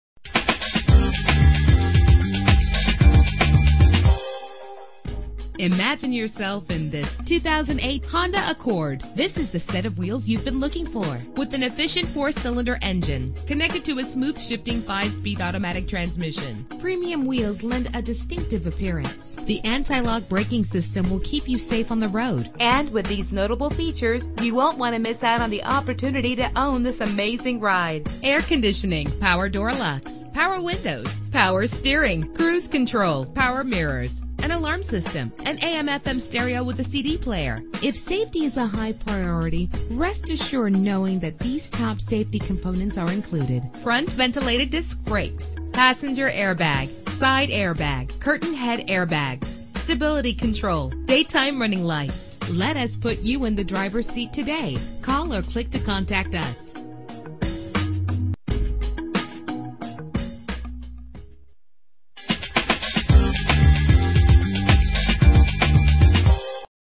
2008 Honda Accord - Old Bridge NJ
Year: 2008
Make: Honda
Model: Accord
Trim: 2.4 LX-S
Engine: 2.4 liter inline 4 cylinder 16 valve
Transmission: 5-Speed Automatic
Color: Blue
Mileage: 20987
Address:
1101 US highway 9